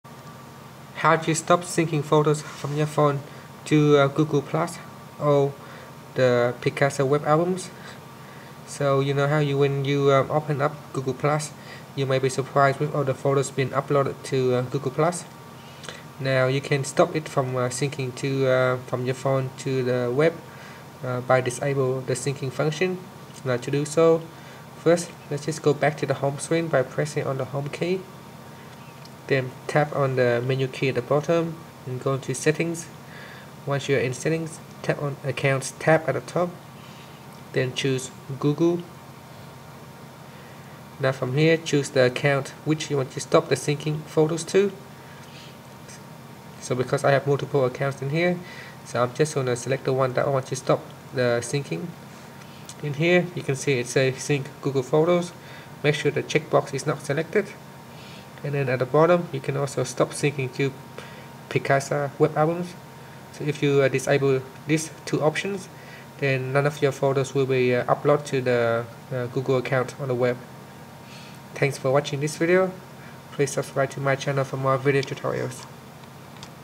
Samsung Galaxy S4: How to Stop Photo Sync From Phone to Google Plus or Picasa Web Album
Learn how you can stop your Samsung Galaxy S4 from uploading photo to Google Plus or Picasa Web album.